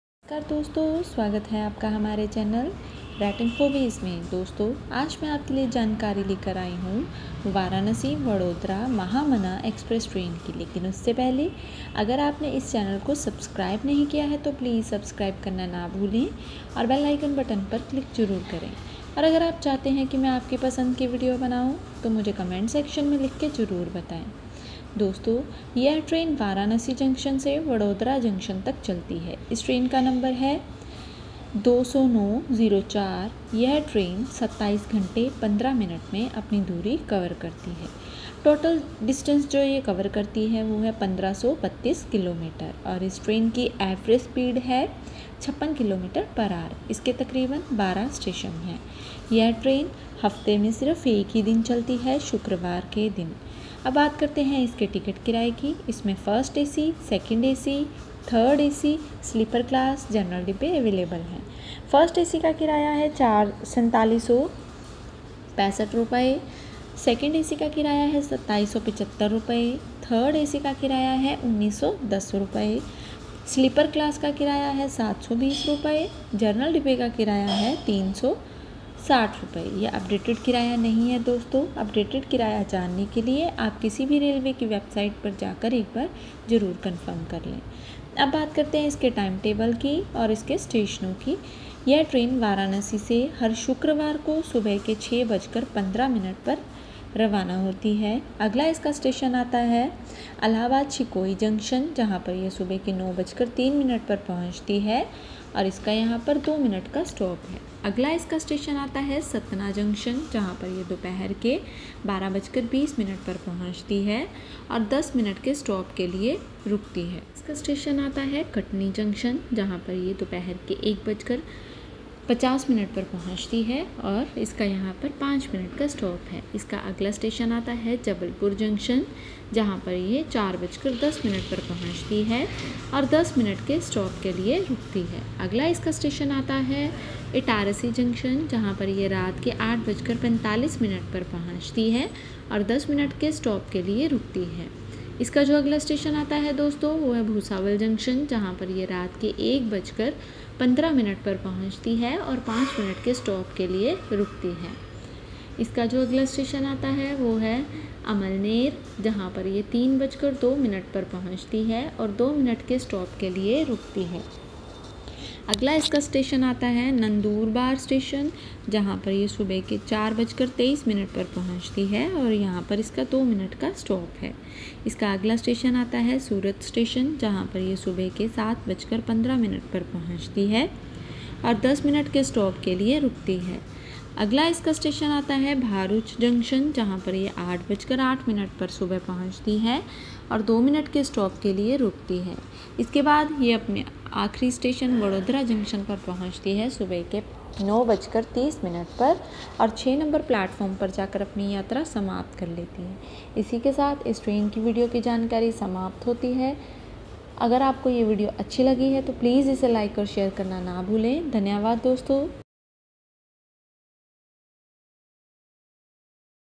20904 train info | Mahamana Express | Indian Railways | Varanasi to Vadodara | वाराणसी से वडोदरा
This video show information on Indian Railways. 20904 train info | Mahamana Express | Indian Railways | Varanasi to Vadodara | वाराणसी से वडोदरा